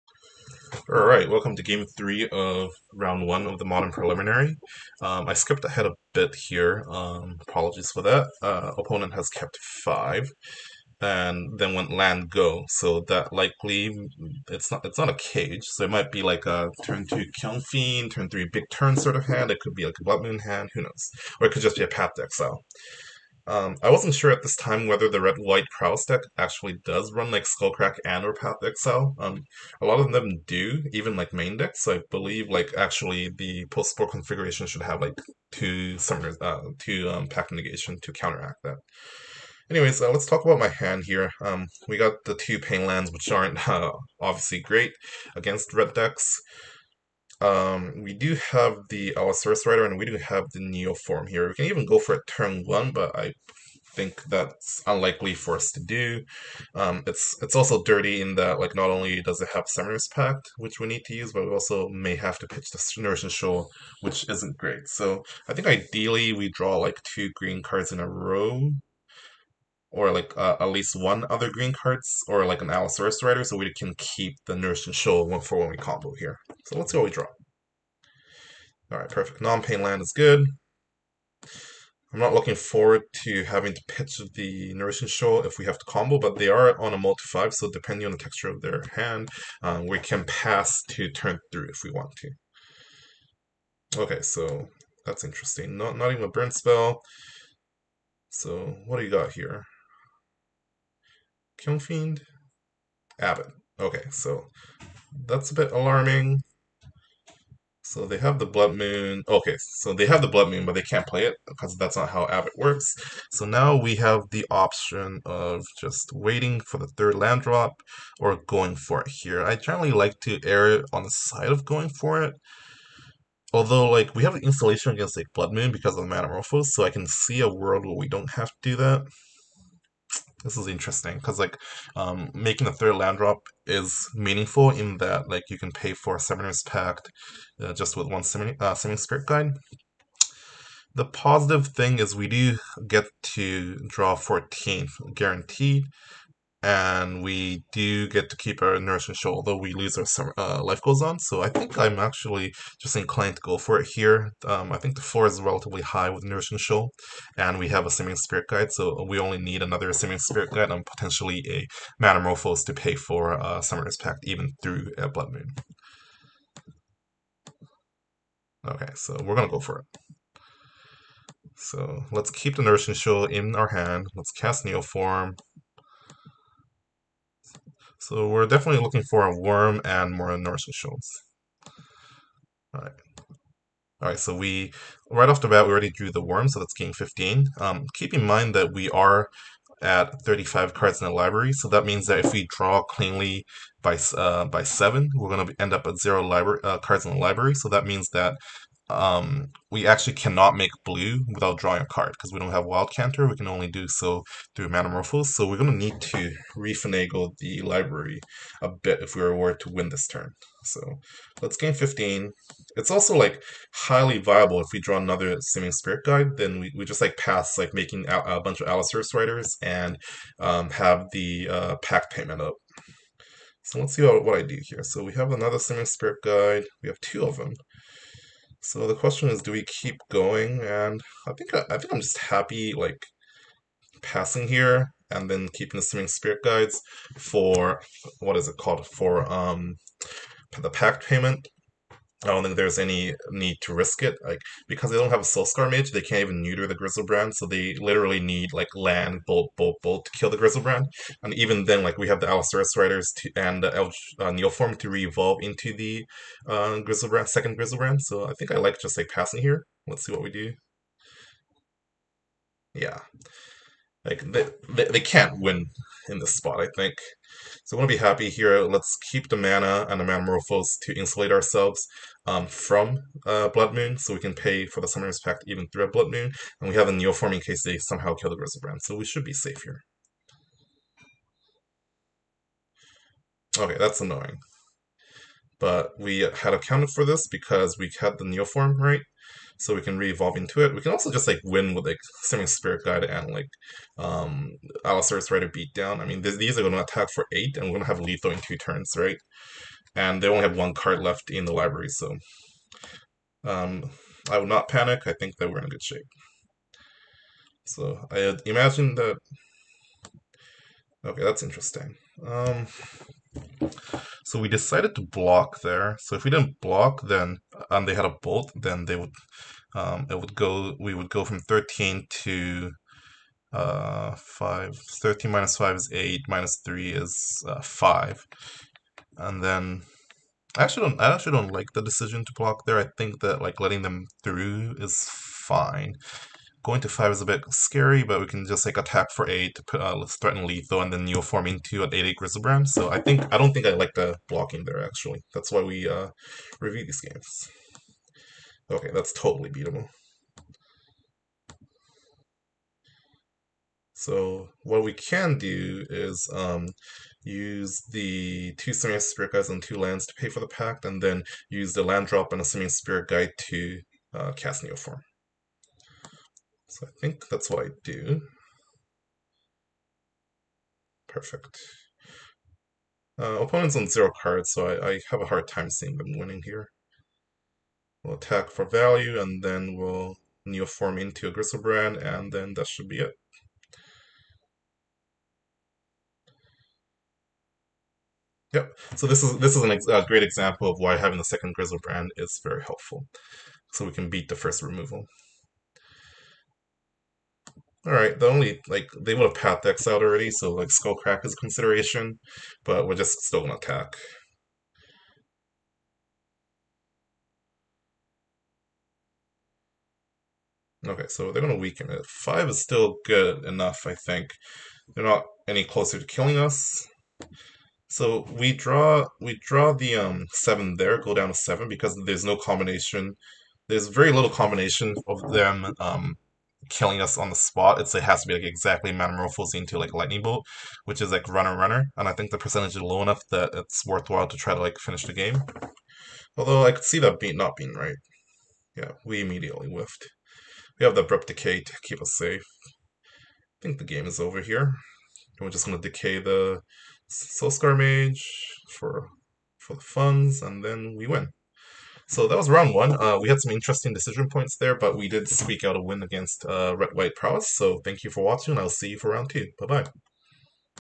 Play & Explain: Neobrand R1G3
Jonathan Zhang (@finalnub) goes through a Modern Preliminary with Neobrand. This is video 4 of 14.